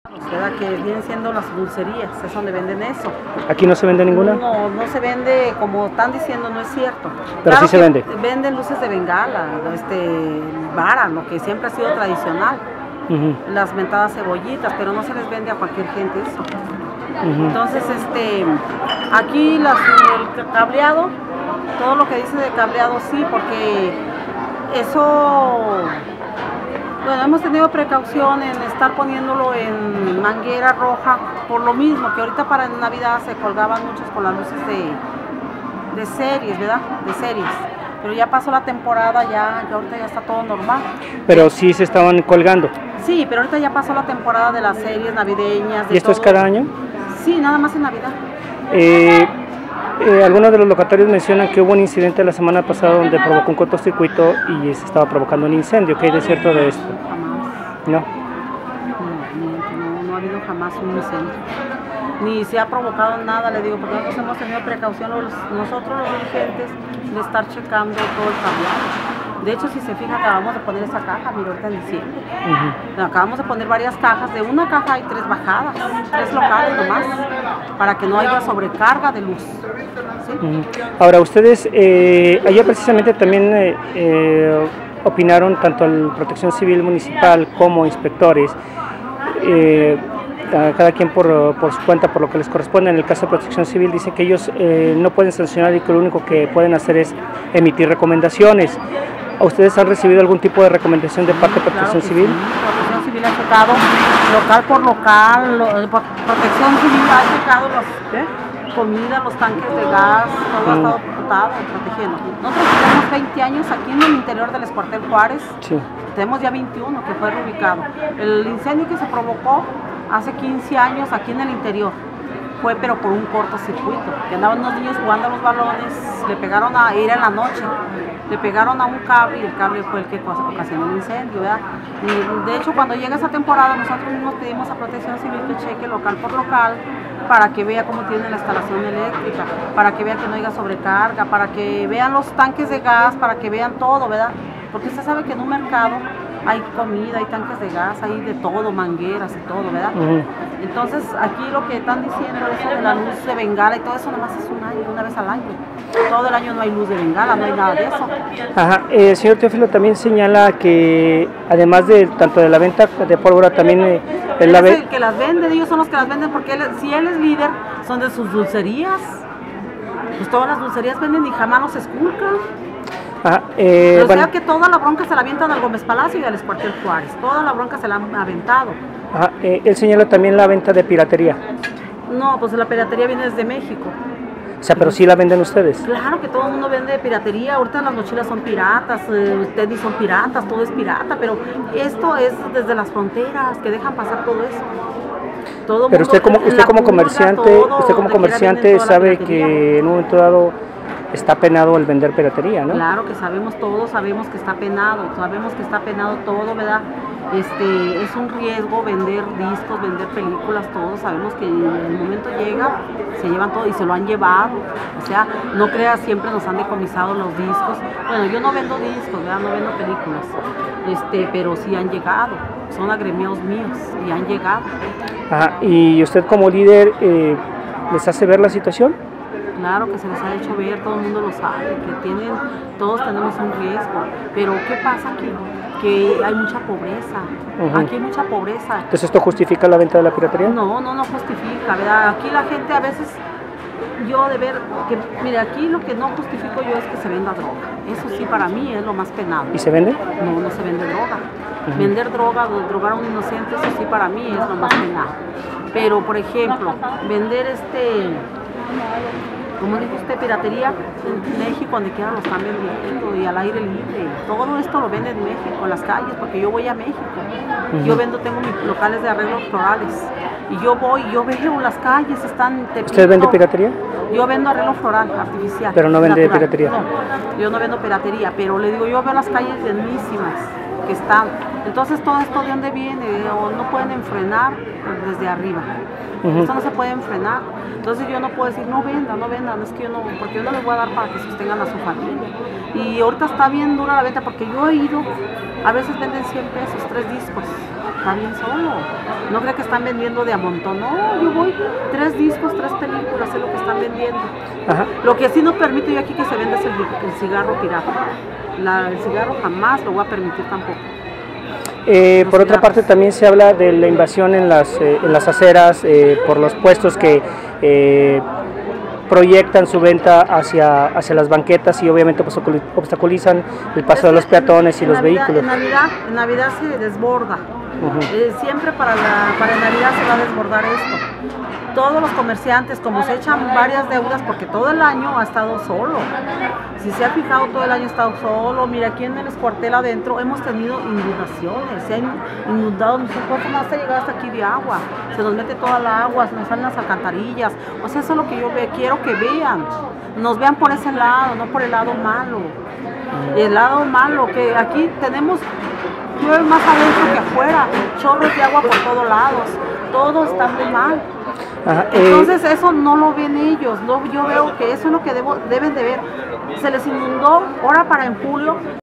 "La pobreza de la gente hace que se venda piratería", Julia García Contreras, Ex Cuartel Juárez
Julia García Contreras, líder de poco mas de 200 comerciantes en el exterior del Excuartel Juárez, reconoce se vende pólvora y artículos piratas en ese lugar."La pobreza de la gente hace que se venda piratería", aseguró la lideresa.